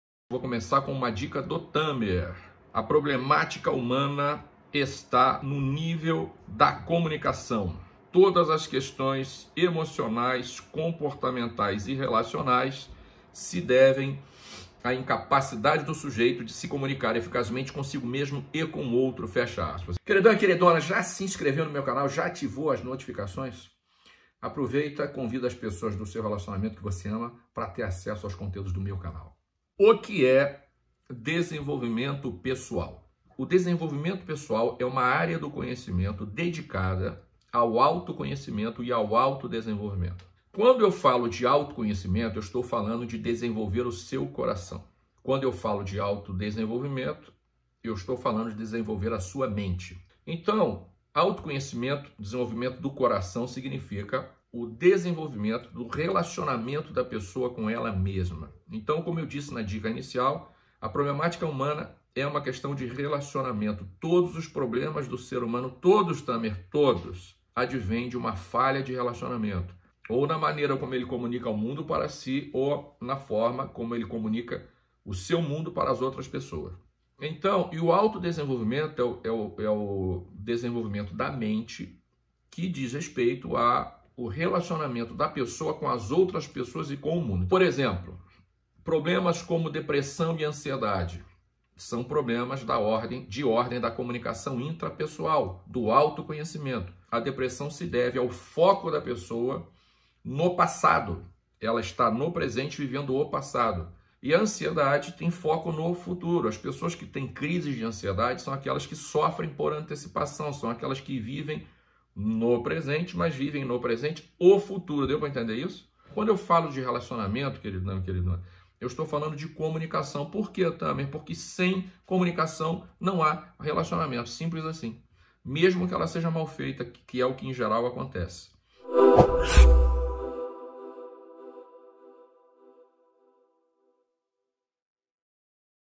Desenvolvimento pessoal: O que é desenvolvimento pessoal - Por Valter Tamer
Por que as pessoas se preparam tanto academicamente, estudam tantos anos, dedicam tantos outros a se preparar para uma carreira e não dedicam o mesmo tempo e esforço no seu desenvolvimento pessoal?

Todos os problemas do ser humano se devem a falhas de comunicação. Os problemas advêm da maneira como o ser humano comunica o mundo para si ou de como ele comunica o seu mundo para as outras pessoas.

O Desenvolvimento Pessoal é uma área do conhecimento dedicada ao autoconhecimento e ao autodesenvolvimento. Autoconhecimento é o desenvolvimento do seu coração e autodesenvolvimento é o desenvolvimento da sua mente.

Desenvolvimento pessoal é algo muito sério. Não é mágica, não é algo sobrenatural, precisa de um movimento da pessoa. É simples trilhar o caminho do desenvolvimento pessoal, mas não é fácil.

É um processo e exige dedicação, tempo, esforço e disciplina. A vida requer preparação e treinamento.

O autoconhecimento e autodesenvolvimento é o único caminho para uma vida plena, a única forma você encontrar o seu lugar no tabuleiro da vida.

Deixe seu comentário, recomende este vídeo para alguém que pode se beneficiar deste conteúdo. Não deixe de se inscrever neste canal, onde compartilho novos conteúdos, toda semana!

Recomende!